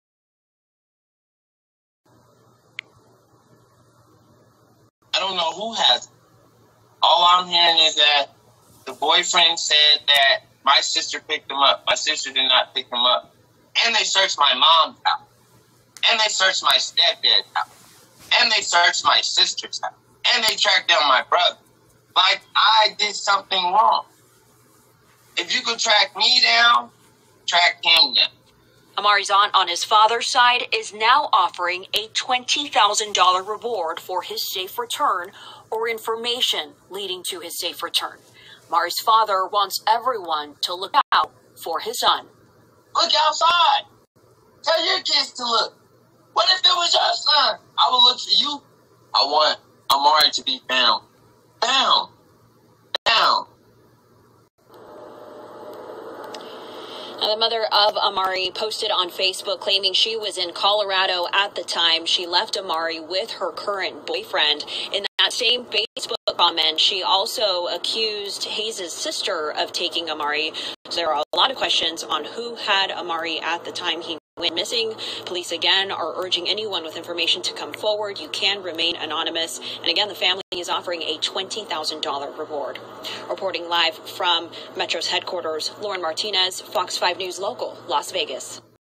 Biological father of Amari Nicholson JyrgioHayes ~(Clip before baby was found)
Biological father of Amari Nicholson ~ Jyrgio Hayes ~News clip (before baby was found)